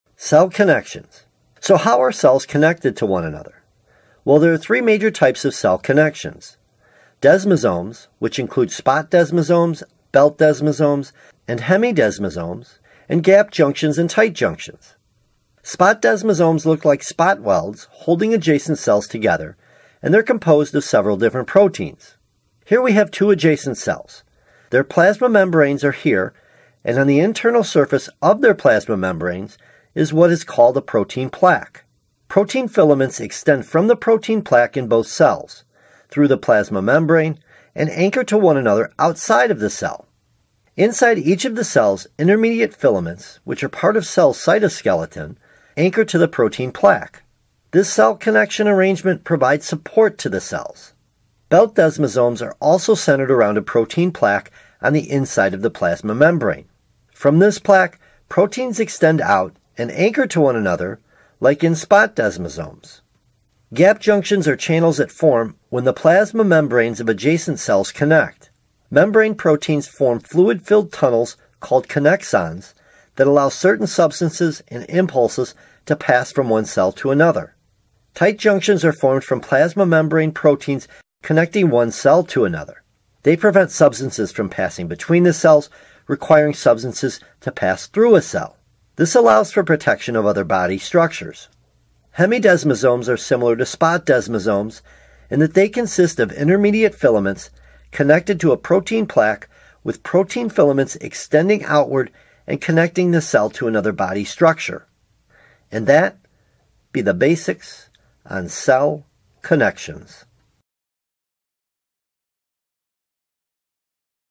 Cell Connections - How Are Cells Connected - What Are Cell Junctions - Types Of Cell Connections
In this video we discuss how are cells connected to one another, as we cover the types of connections cells have to one another.

Transcript (with notes)

Cell connections
How are cells connected to one another?  Well, there are 3 major types of cell connections, desmosomes, which include spot desmosomes, belt desmosomes and hemidesmosomes, gap junctions and tight junctions.

Spot Desmosomes look like spot welds holding adjacent cells together, and they are composed of several different proteins.  Here we have two adjacent cells.  Their plasma membranes are here, and on the internal surface of their plasma membranes is what is called a protein plaque.

Protein filaments extend from the protein plaque in both cells, through the plasma membrane and anchor to one another outside of the cell.  Inside each of the cells, intermediate filaments, which are part of a cells cytoskeleton, anchor to the protein plaque.  This cell connection arrangement provides support to the cells.

Belt desmosomes are also centered around a protein plaque on the inside of the plasma membrane.  From this plaque, proteins extend out and anchor to one another like in spot desmosomes.
Gap junctions are channels that form when the plasma membranes of adjacent cells connect.

Membrane proteins form fluid filled tunnels called connexons, that allow certain substances and impulses to pass from one cell to another.

Tight junctions are formed from plasma membrane proteins connecting one cell to another.  They prevent substances from passing between cells requiring substances to pass through a cell.  This allows for protection of other body structures.

Hemidesmosomes are similar to spot desmosomes in that they consist of intermediate filaments connected to a protein plaque with protein filaments extending outward and connecting the cell to another body structure.

Timestamps
0:00 The 3 major cell connections
0:16 Spot desmosomes
0:51 Belt desmosomes
1:03 Gap junctions
1:18 Tight junctions
1:31 Hemidesmosomes